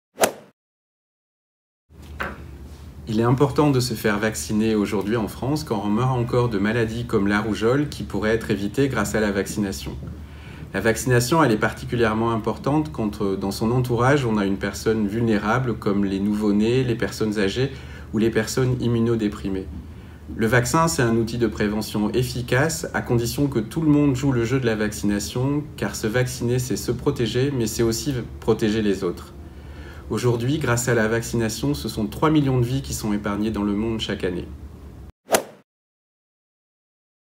Pourquoi se faire vacciner ?
Interview de Fabrice Meillier du Comité Vaccins du Leem